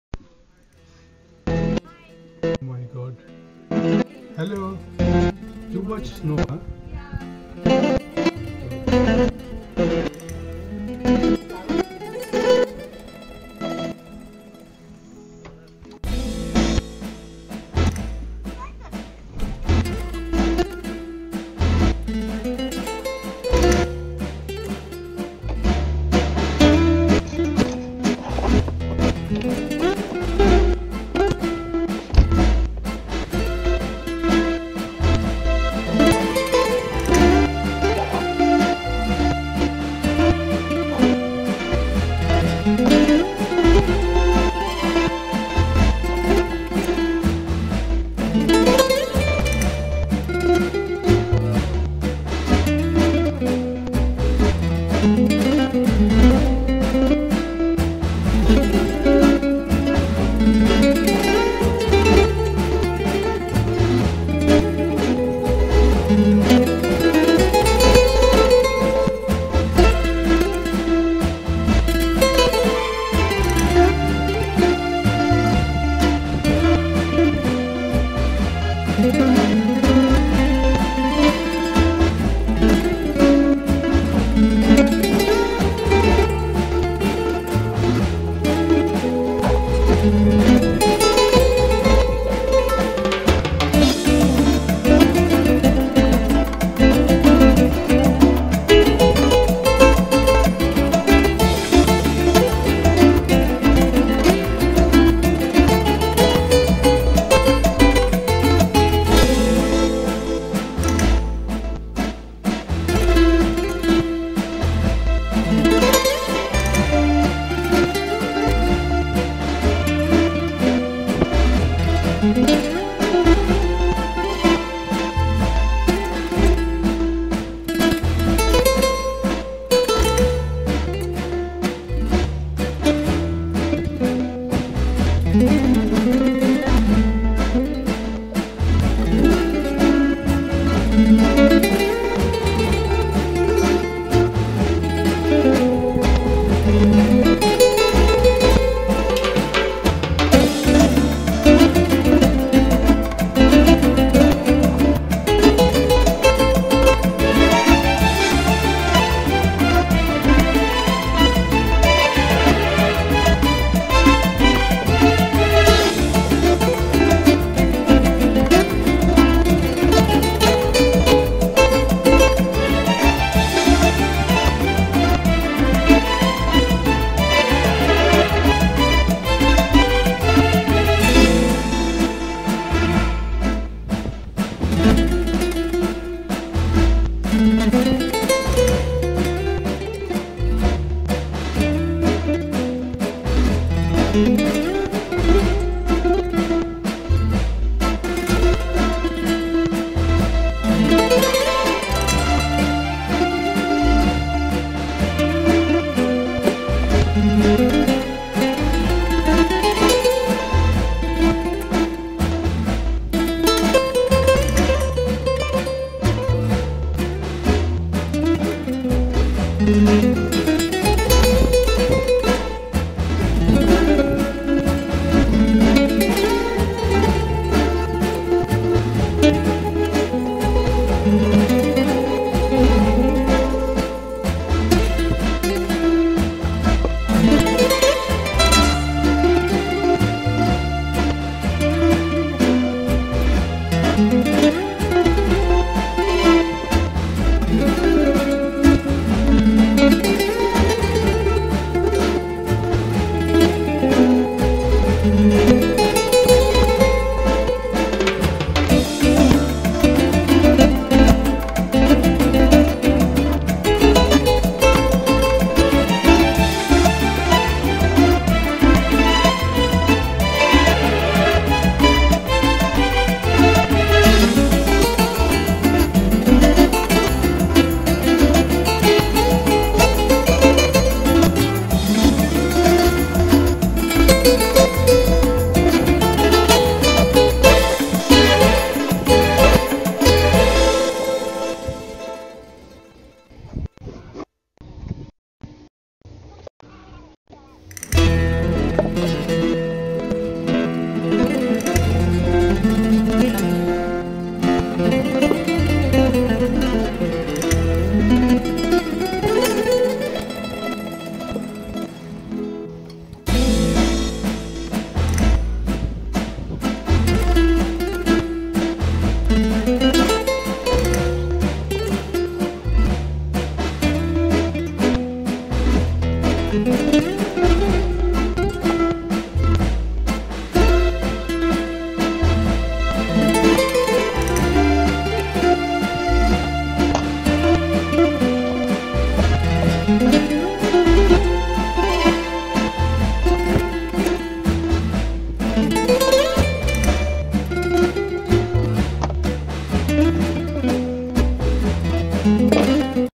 Calgary Snow Storm 22 - 22 December 2020
We had a snow storm in the Southern Alberta on the 21 - 22 December night and morning. 20 - 40 cm snow was dumped in little more than 12 hours. When we opened the door in the morning. got a shock seeing the amount of snow. Blizzard like winds pushed large amount if snow near our front door such that it was little hard to open the door. Here is a collection of video footage from our security camera. I couldn’t speed up the video not more than X2 times. It is a document which could be boring. But Spanish Flamenco background music could help to pass the time.